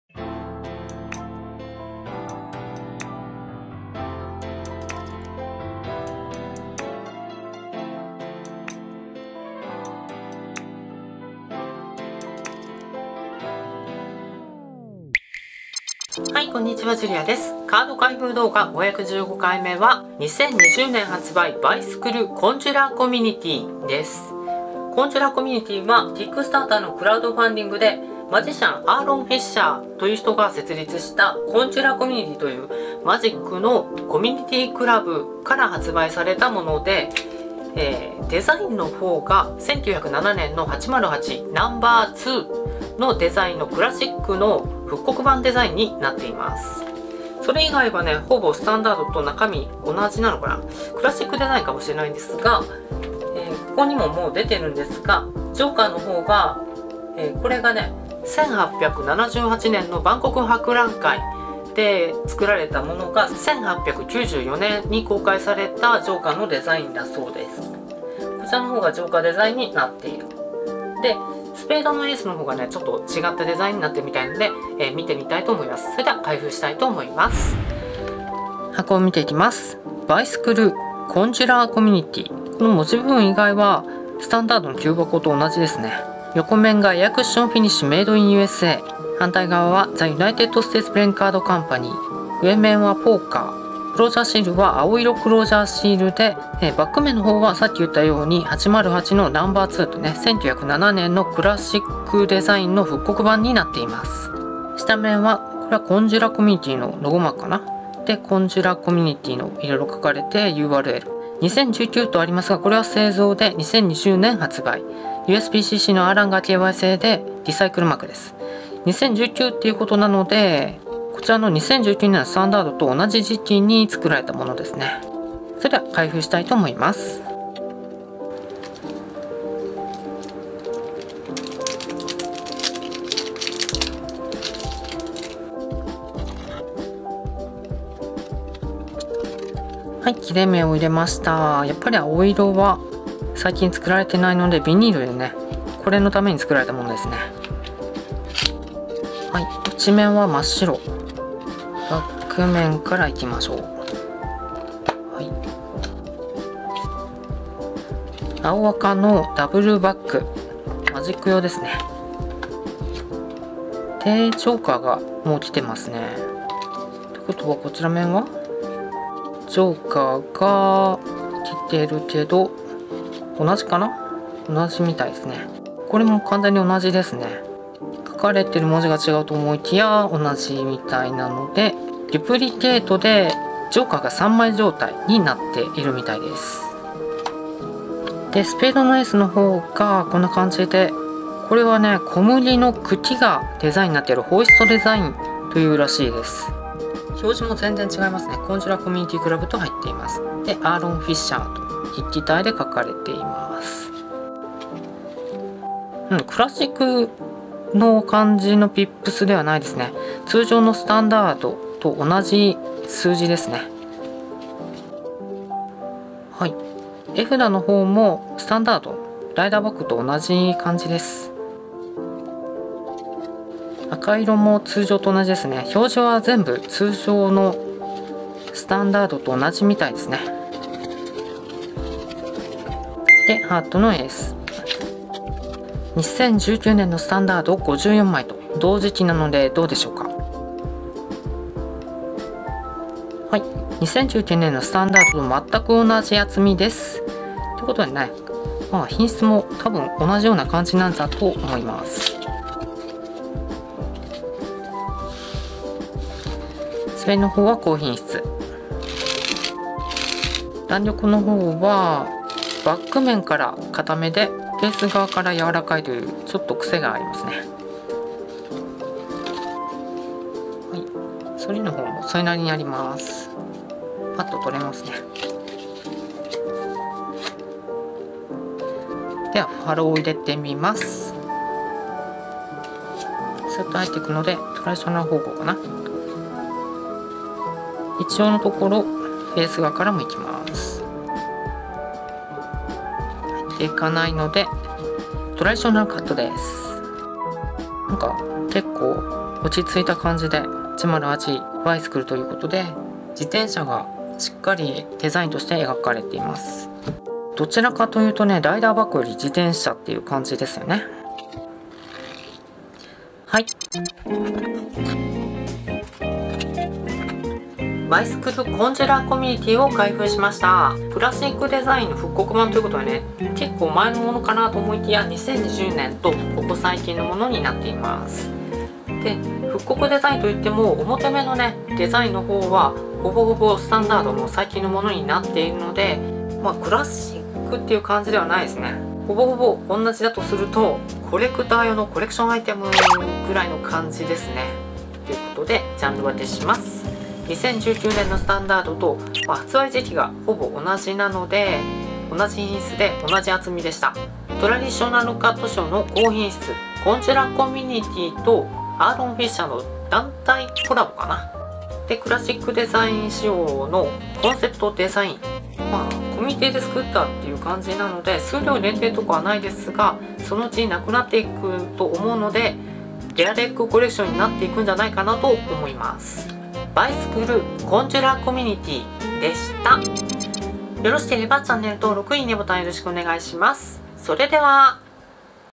カード開封動画vol 515バイスクルコンジュラコミュニティ
マジシャン「Aaron Fisher/アーロンフィッシャー」が設立したマジッククラブ「CONJUROR COMMUNITY CLUB」で発売した

バイスクルです(*'ω'*)

バックデザインは1907年の808 No.2の復刻デザイン('ω')ノジョーカーが1878年の万国博覧会用で1894年に公開されたデザイン

スペードAは小麦の茎のホイストデザインになっています('ω')ノ

日本ではほとんど出回っていないため一度見失うとかなり入手困難になる部類の普通のバイスクルです(苦笑)

バイシクル・デック：モーデルNo.2復刻（レッド）Bicycle Deck: CC Club (Model No.2 Reprint) (プレイフェア)

動画内BGM